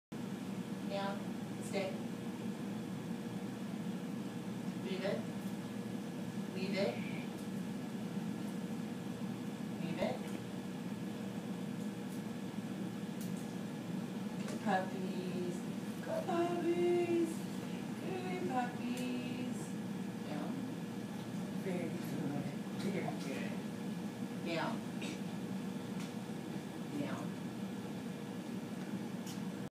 German Shepherds working on down stay
Working 2 German Shepherds in down stays.
One is being used as a "demo" dog to help train the other.

Sable German Shepherd, Dog tricks, positive reinforcement, dog training, dog behavior, animal tricks
Professional Dog Trainer
Lisa Shipley
K9 Karma Dog Training